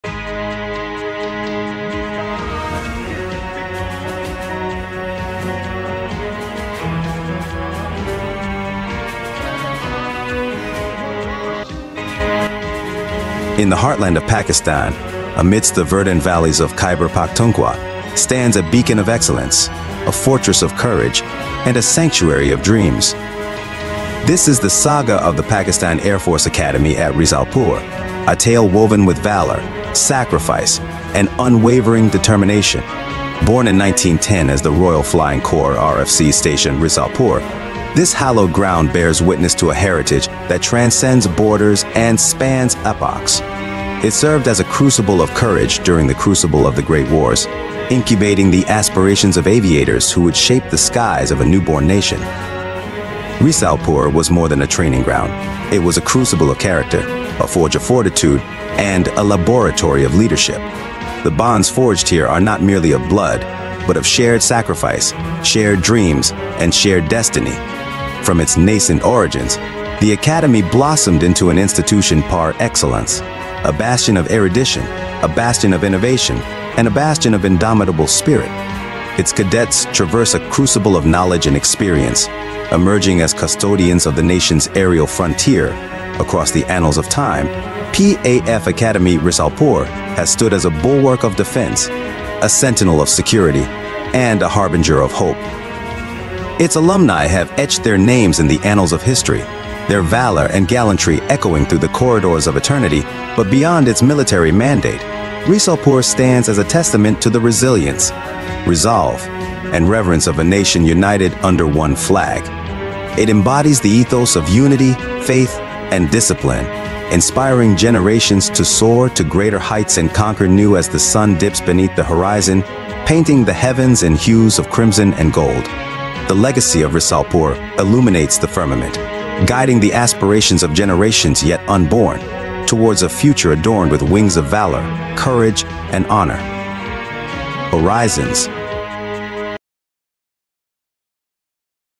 Inside PAF Academy ⚔️Risalpur || PAF Academy Risalpur Documentary 2024
credit google
The Pakistan Air Force Academy Asghar Khan is an accredited three-year military academy which provides undergraduate education to officer candidates for the Pakistan Air Force. The eligible and selected candidates from all over Pakistan are sent to PAF Academy Risalpur for flying training. Wikipedia
Motto: فقط ذوقِ پرواز ہے زندگى (Urdu)
Administrative staff: 400
AOC: AVM Ghazanfar latif GD(P) SI(M)
Established: 1910; 114 years ago
Undergraduates: 1000 (approx.)
Subsidiary: College of Flying Training